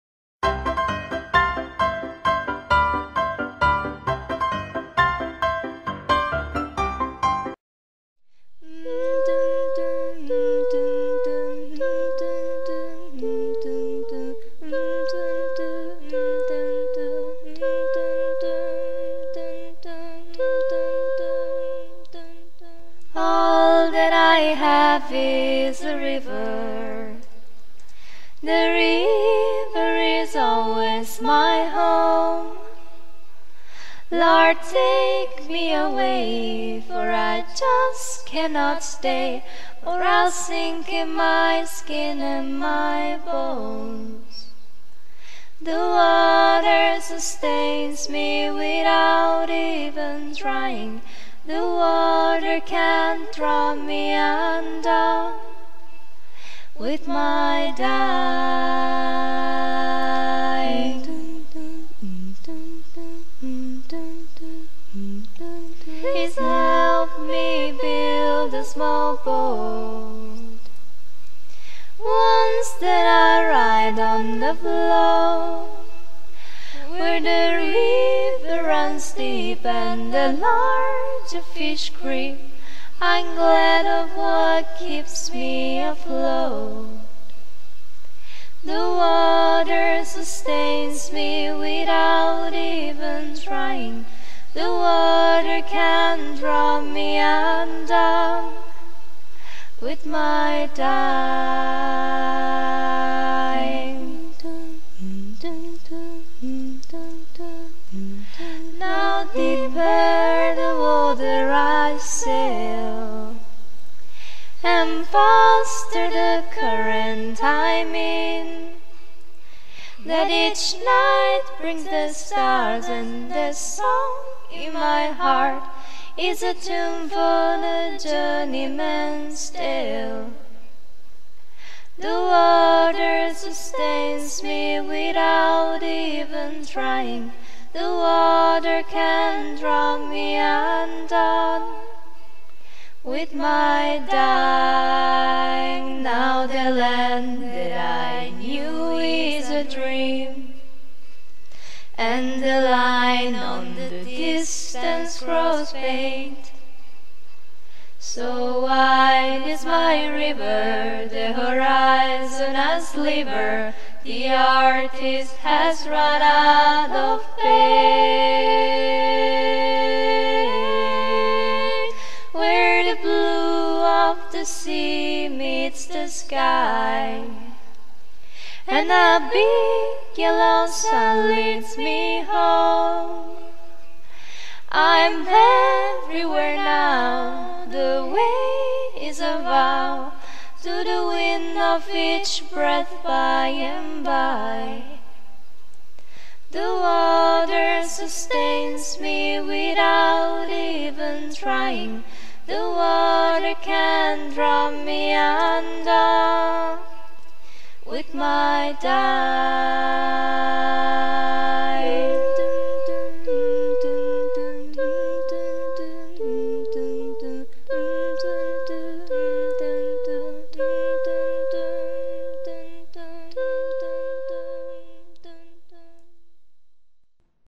The Water - Speed Painting
I used Photoshop. Done in 1 hour and 45 minutes.